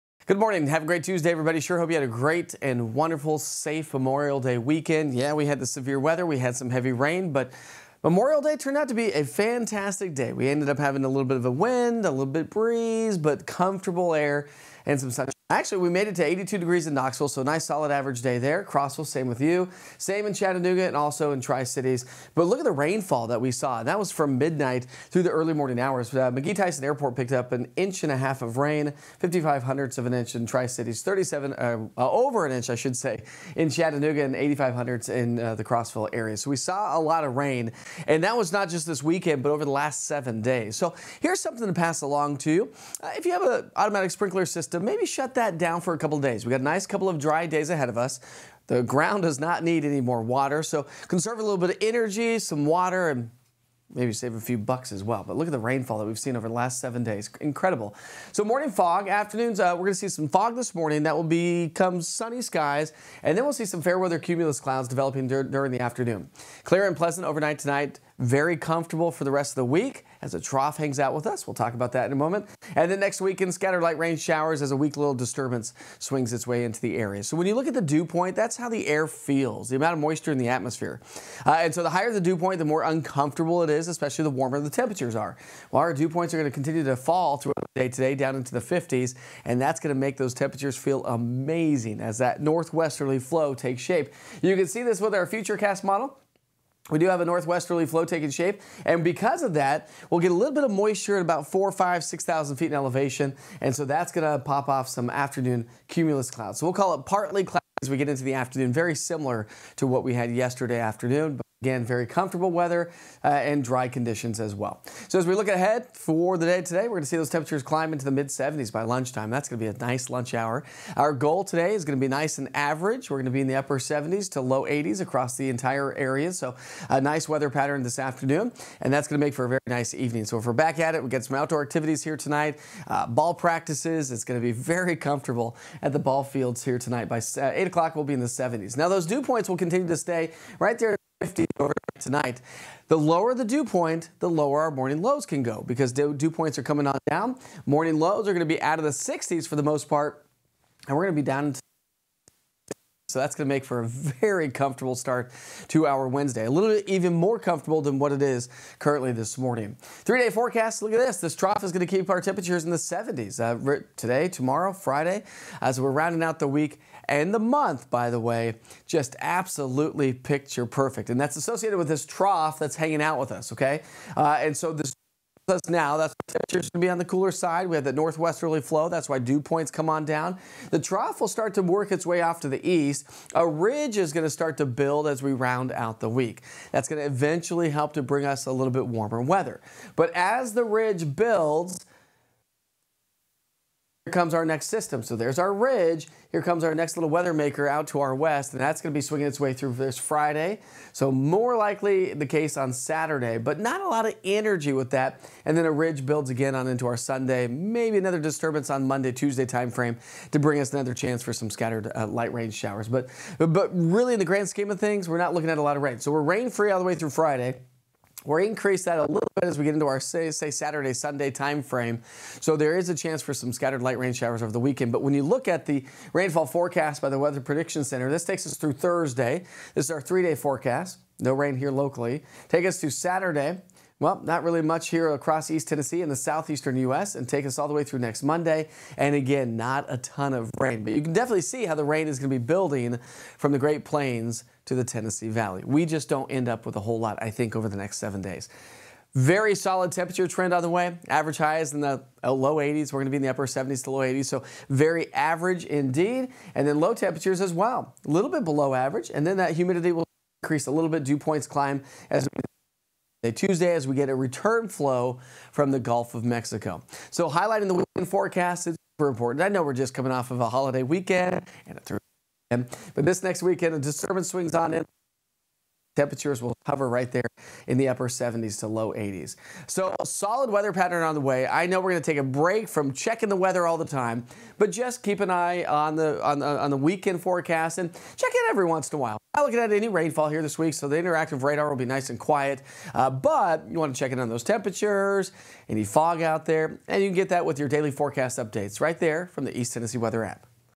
6am - Tuesday, May 28, 2024
Well.. Memorial Day turned out to be a fantastic day! Now, drier air settles in, leaving us with average and pleasant weather. But, how long does it last? Here's the rest of the story with Meteorologist Mike Witcher.